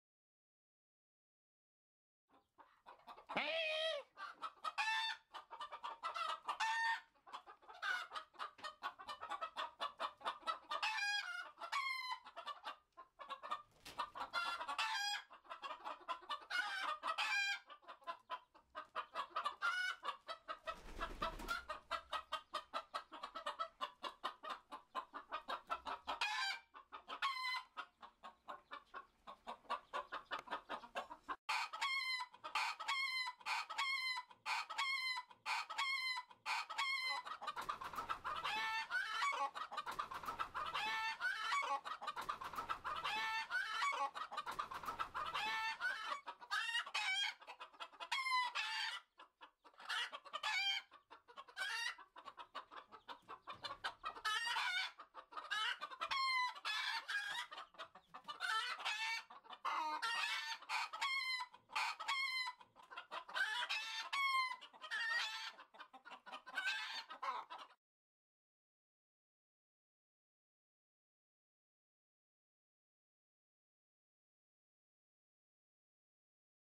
funny chicken escape away from home
funny and sweet chickens running away from a big bully father :D phaha